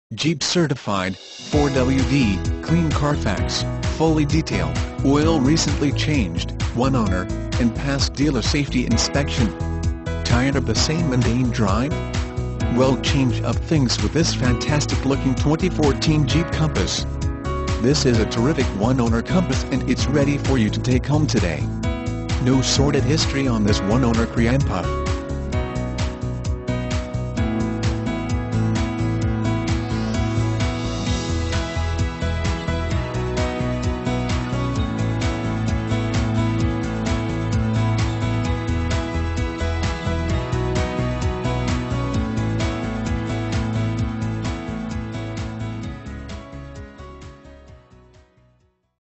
2014 JEEP COMPASS Torrington, CT 10110
2014 JEEP COMPASS Torrington, CT
Stock# 10110

Sullivans Northwest Hills Dealership
2065 E. Main St

Jeep Certified, 4WD, CLEAN CARFAX, FULLY DETAILED, OIL RECENTLY CHANGED, ONE OWNER, and PASSED DEALER SAFETY INSPECTION. Tired of the same mundane drive? Well change up things with this fantastic-looking 2014 Jeep Compass. This is a terrific one-owner Compass and it's ready for you to take home today. No sordid history on this one-owner creampuff.